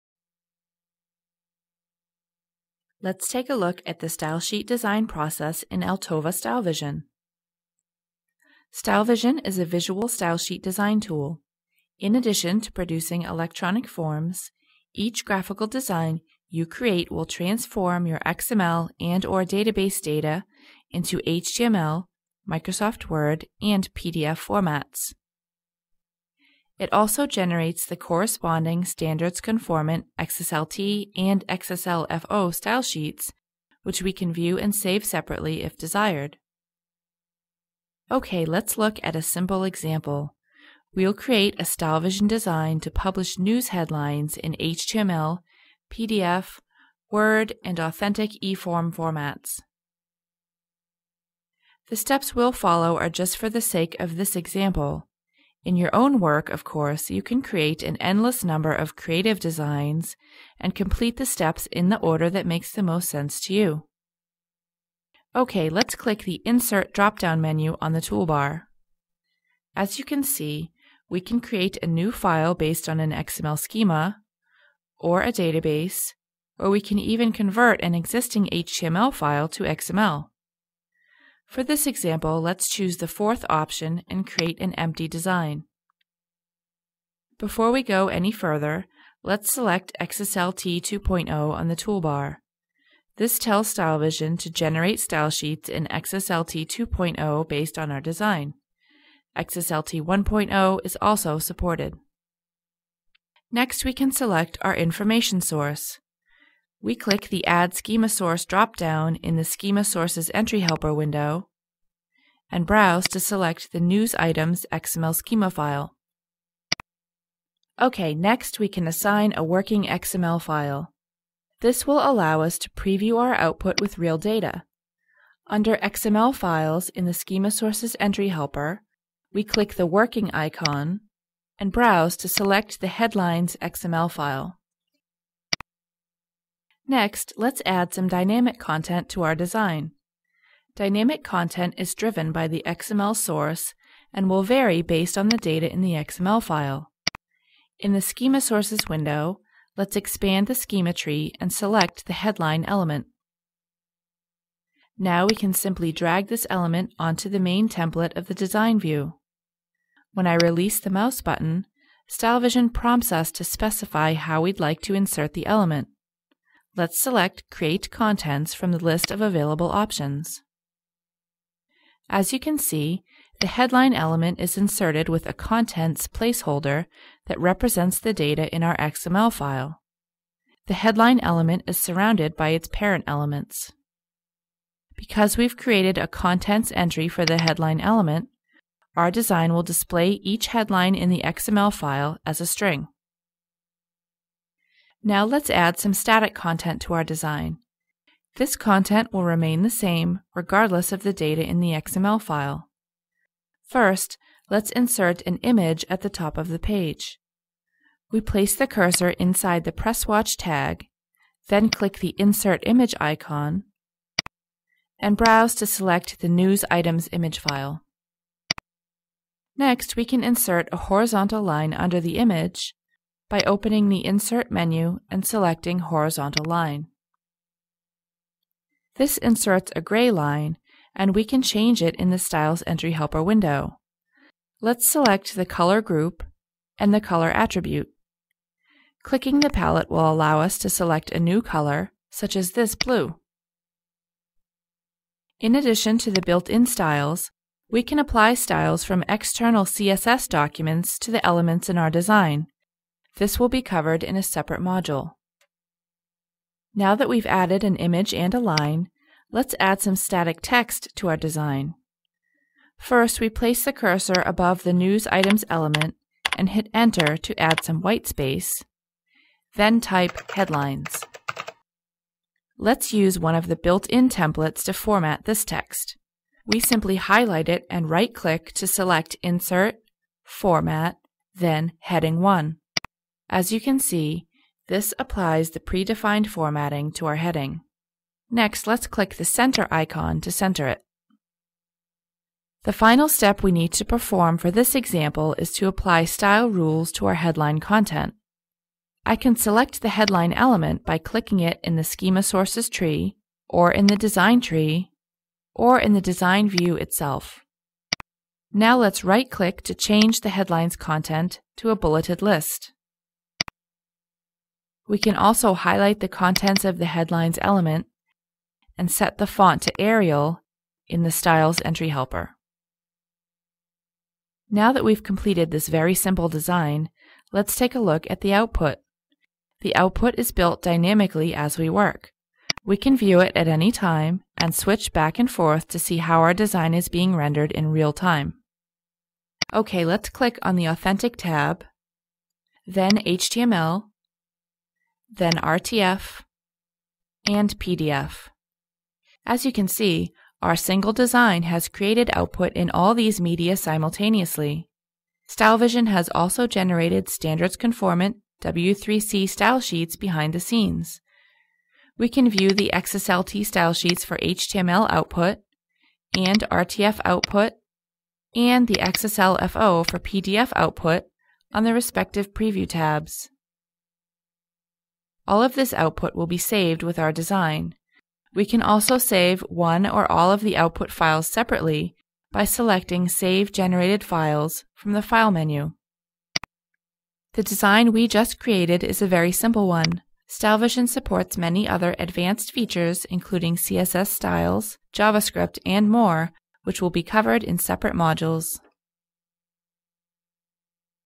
Visual Stylesheet Design
Learn about drag & drop XSLT stylesheet design using StyleVision. Publish XML and database data in HTML, PDF, and MS Word.